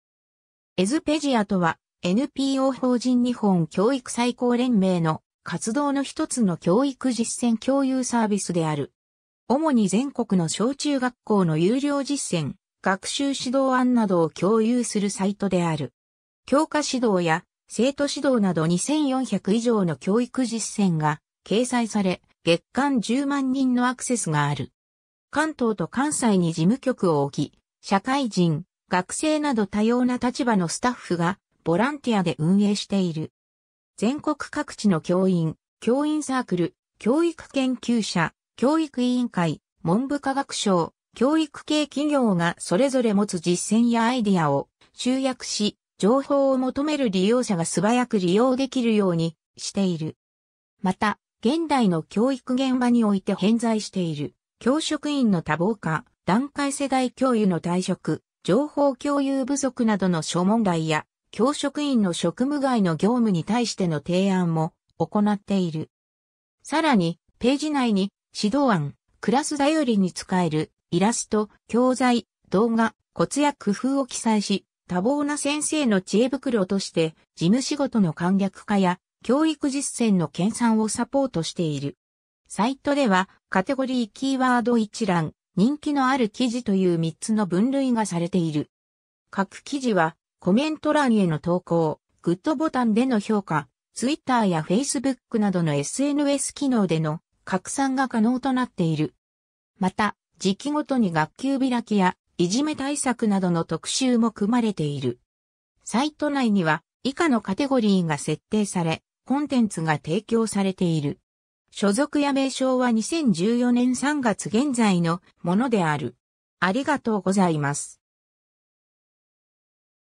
EDUPEDIA
EDUPEDIA（エデュペディア）とは、NPO法人日本教育再興連盟（ROJE）の活動のひとつの教育実践共有サービスである。
主に全国の小中学校の優良実践、学習指導案などを共有するサイトである。
教科指導や生徒指導など2400以上の教育実践が掲載され、月間10万人のアクセスがある（2017年現在）。
関東と関西に事務局をおき、社会人、学生など多様な立場のスタッフがボランティアで運営している。
 全国各地の教員・教員サークル・教育研究者・教育委員会・文部科学省・教育系企業がそれぞれ持つ実践やアイディアを集約し、情報を求める利用者が素早く利用できるようにしている。
また、現代の教育現場において遍在している、教職員の多忙化・団塊世代教諭の退職・情報共有不足などの諸問題や教職員の職務外の業務に対しての提案も行っている。
さらにページ内に、指導案・クラス便りに使えるイラスト・教材・動画・コツや工夫を記載し、多忙な先生の知恵袋として事務仕事の簡略化や、教育実践の研鑽をサポートしている。
 サイトでは、カテゴリー（下記参照）・キーワード一覧・人気のある記事という3つの分類がされている。
各記事はコメント欄への投稿、「GOODボタン」での評価、twitterやfacebookなどのSNS機能での拡散が可能となっている。
また、時期ごとに学級開きやいじめ対策などの特集も組まれている。
 サイト内には以下のカテゴリーが設定され、コンテンツが提供されている。
 （評価・閲覧度が高い記事から引用） ※所属や名称は2014年3月現在のものである。